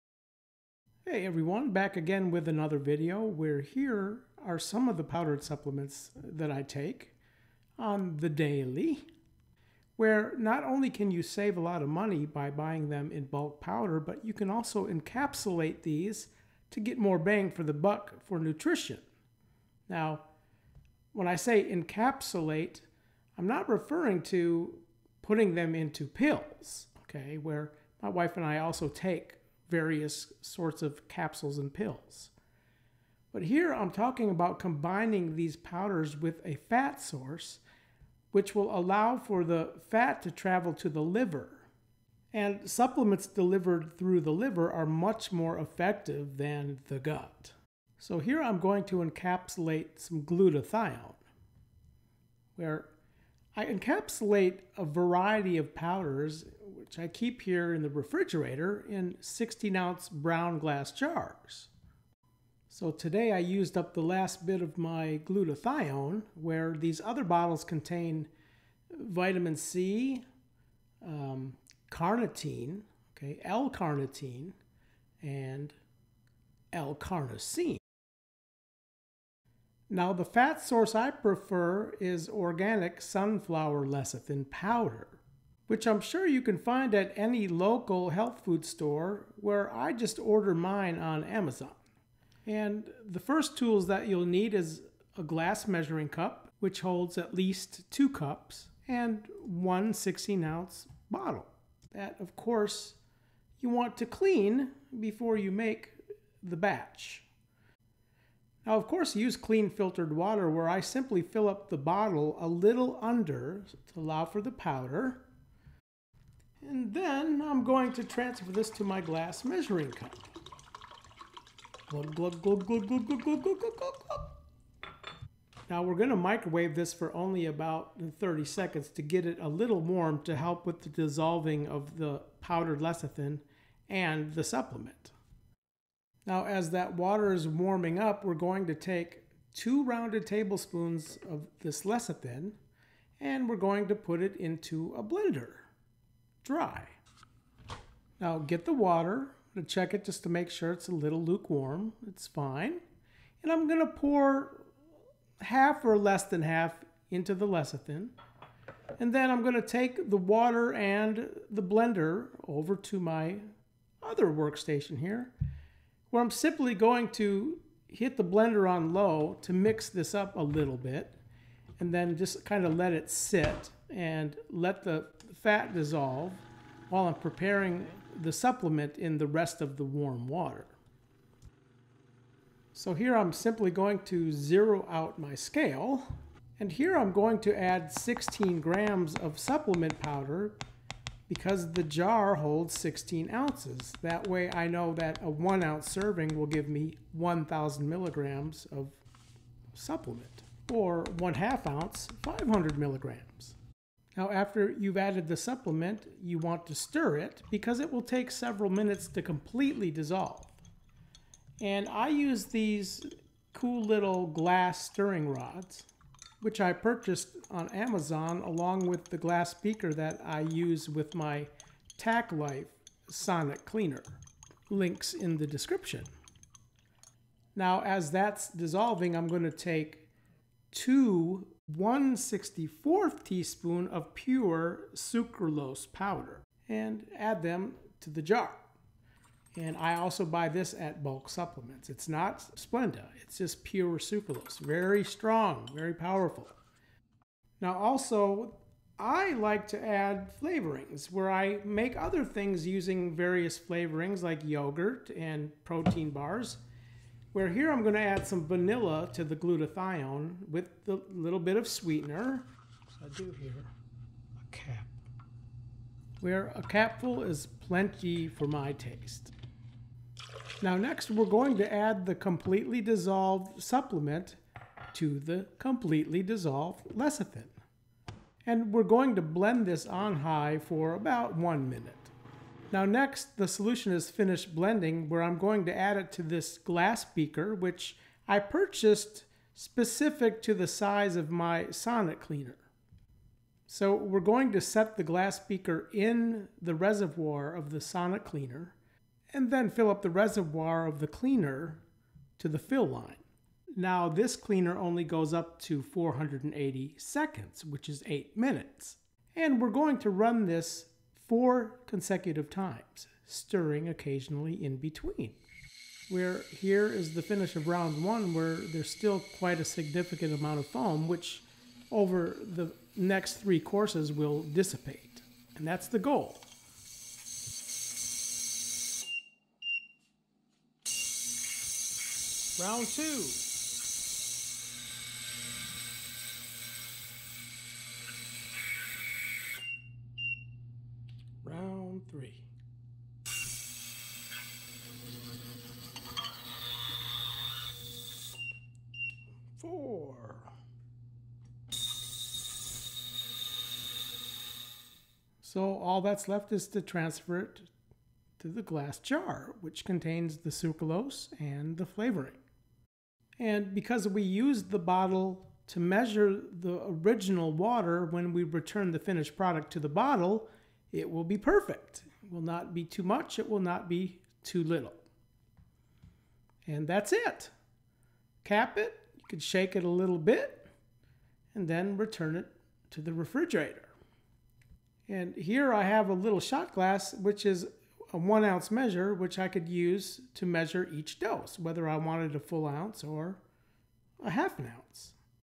How To Make "Liposomal" Vitamin C (Disclaimer Below)
However, this method of "sonication" creates an encapsulated "emulsion" which is not 100 percent bioavailable like liposomes BUT it still creates a much higher bioavailability than plain C.

Bulksupplements.com